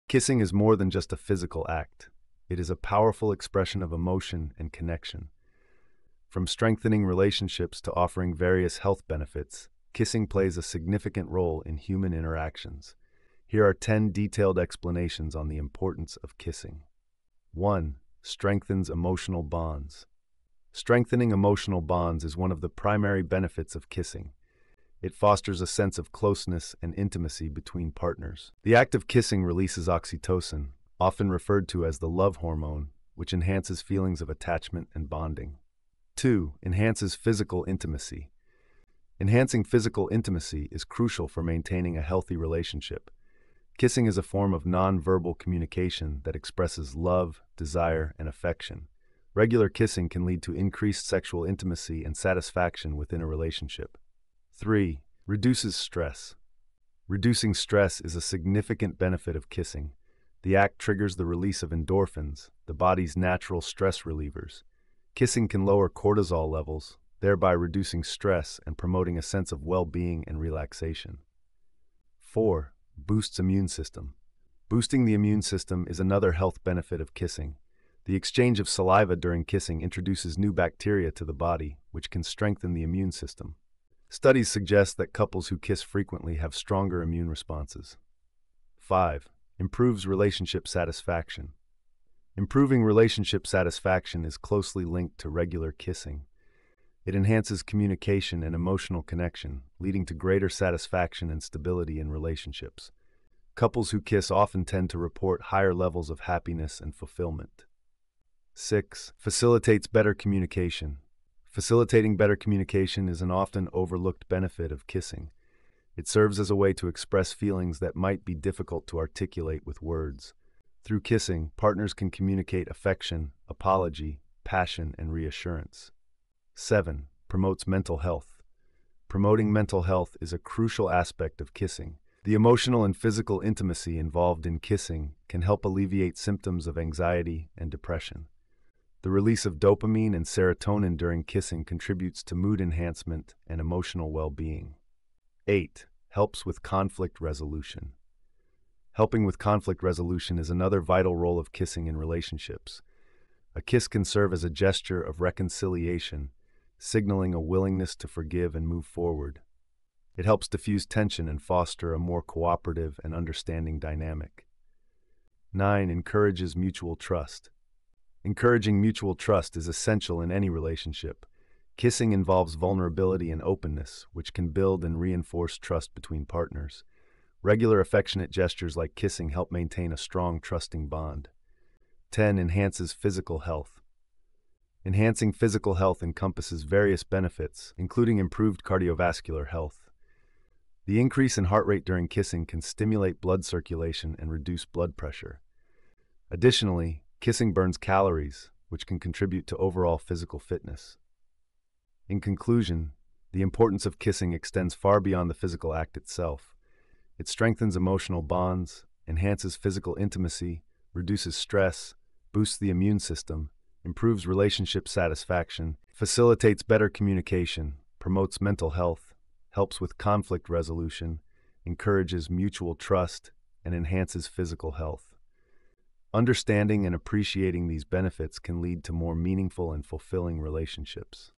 10 Explanations on The Importance of Kissing 💕
Transcript:
(00:00) kissing is more than just a physical act it is a powerful expression of emotion and connection from strengthening relationships to offering various health benefits kissing plays a significant role in human interactions here are 10 detailed explanations on the importance of kissing one strengthens emotional bonds strengthening emotional bonds is one of the primary benefits of kissing it Fosters a sense of closeness and intimacy between partners the act of kissing releases oxytocin often referred to as the love hormone which enhances
(00:34) feelings of attachment and bonding two enhances physical intimacy enhancing physical intimacy is crucial for maintaining a healthy relationship kissing is a form of non-verbal communication that expresses love desire and affection regular kissing can lead to increased sexual intimacy and satisfaction within a relationship three reduces stress reducing stress is a significant benefit of kissing the ACT triggers the release of endorphins the body's natural stress relievers kissing can lower cortisol levels thereby reducing stress and
(01:09) promoting a sense of well-being and relaxation four boosts immune system boosting the immune system is another health benefit of kissing the exchange of saliva during kissing introduces new bacteria to the body which can strengthen the immune system studies suggest that couples who kiss frequently have stronger immune responses five improve relationship satisfaction improving relationship satisfaction is closely linked to regular kissing it enhances communication and emotional connection leading to Greater satisfaction and
(01:41) stability in relationships couples who kiss often tend to report higher levels of happiness and fulfillment six facilitates better communication facilitating better communication is an often overlooked benefit of kissing it serves as a way to express feelings that might be difficult to articulate with words words through kissing Partners can communicate affection apology passion and reassurance seven promotes mental health promoting mental health is a crucial aspect of kissing the emotional and physical intimacy involved in kissing
(02:15) can help alleviate symptoms of anxiety and depression the release of dopamine and serotonin during kissing contributes to mood enhancement and emotional well-being eight helps with conflict resolution helping with conflict resolution is another vital role of kissing in relationships a kiss can serve as a gesture of reconciliation signaling a willingness to forgive and move forward it helps diffuse tension and Foster a more cooperative and understanding Dynamic nine encourages Mutual trust encouraging Mutual trust is essential in
(02:50) any relationship kissing involves vulnerability and openness which can build and reinforce trust between partners regular affectionate gestures like kissing help maintain a strong trusting Bond 10 enhances physical health enhancing physical health encompasses various benefits including improved cardiovascular health the increase in heart rate during kissing can stimulate blood circulation and reduce blood pressure additionally kissing burns calories which can contribute to overall physical fitness in conclusion the importance of
(03:24) kissing extends far beyond the physical act itself it strengthens emotional bonds enhances physical intimacy reduces stress boosts the immune system improves relationship satisfaction facilitates better communication promotes mental health helps with conflict resolution encourages Mutual trust and enhances physical health understanding and appreciating these benefits can lead to more meaningful and fulfilling relationships

Chosen Title: "Release of Feel-Good Hormones: The Science Behind Kissing"
Reason: This title combines the intrigue of science with the emotional aspect of kissing, likely to attract viewers interested in both aspects.

YOUTUBE DESCRIPTION:
Delve into the fascinating world of kissing and discover its importance through 10 detailed explanations in this enlightening video. Explore how kissing fosters physical and emotional bonds, releases feel-good hormones, and enhances communication and connection in relationships. Uncover the stress-reducing benefits, immunity-boosting effects, and relationship satisfaction that kissing can bring. Learn about the cultural significance and symbolism of kissing, as well as its positive impact on cardiovascular health and overall well-being. Join us on a journey of exploration and understanding as we unravel the importance of kissing in human interactions.